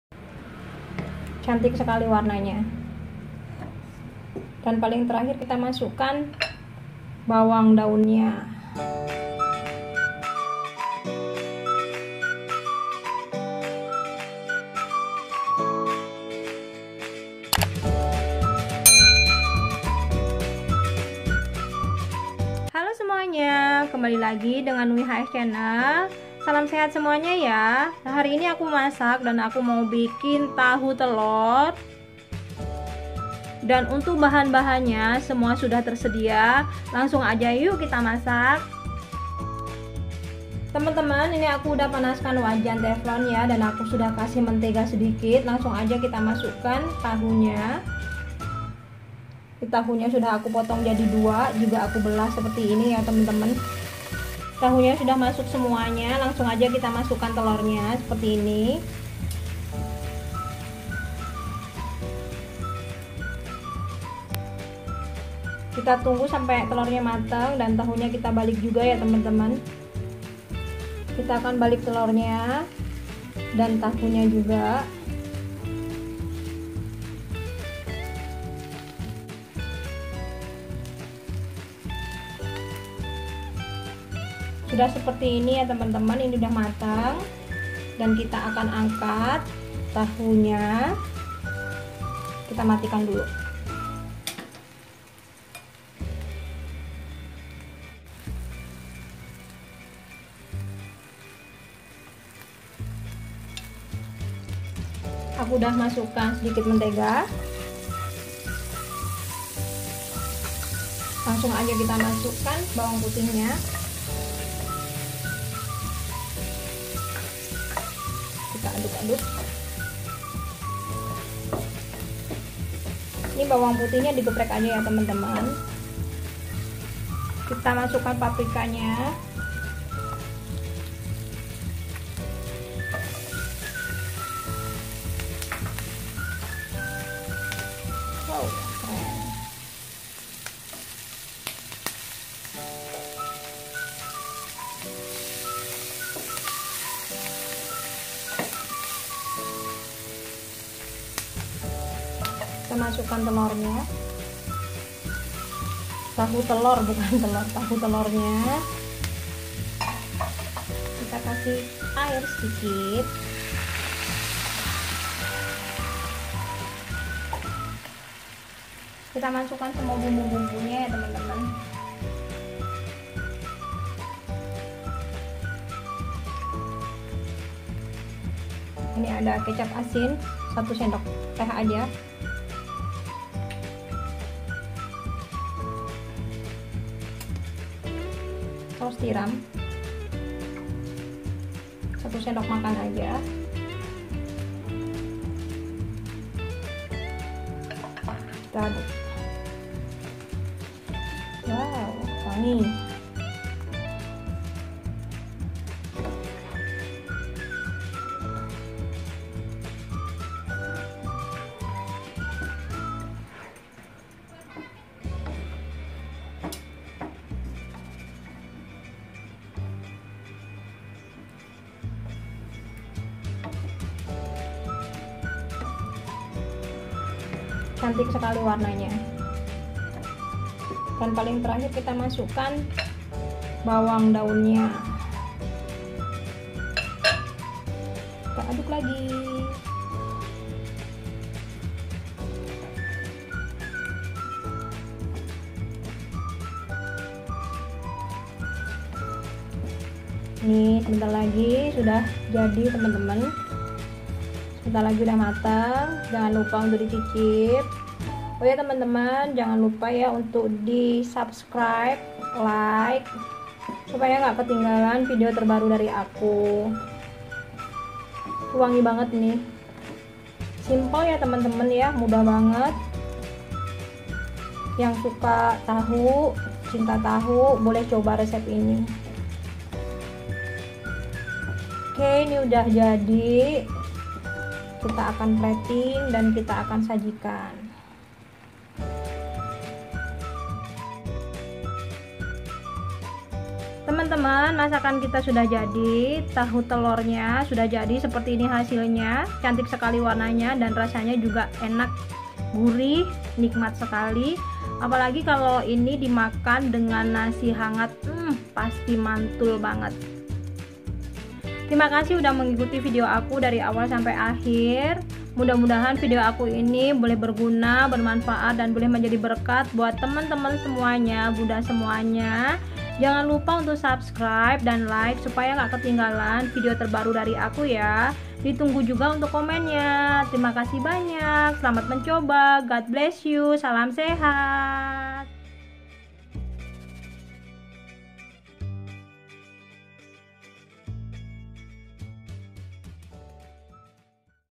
Resep Tahu Telur Mudah & Simple
Salam sehat semuanya..
Masak yuukkk...Temen-temen hari ini aku mau berbagi resep satu menu masakan yang simple dan sangat mudah membuatnya..Nama menunya yaitu Tahu Telur..Bagi temen-temen yang suka sekali dengan tahu wajib coba resep ini...

Bahan2nya terdiri dari:
* 3 bh tahu putih  ( potong jadi dua  dan belah )
* 1 btr telur
* 2 siung bawang putih di geprek
* 1 btg bawang daun
* paprika hijau secukupnya
* paprika merah secukupnya
* 1  sdt kaldu jamur
* 1 sdt  kecap asin
* 1 sdm saus tiram
* 11/2 sdt gula pasir

God bless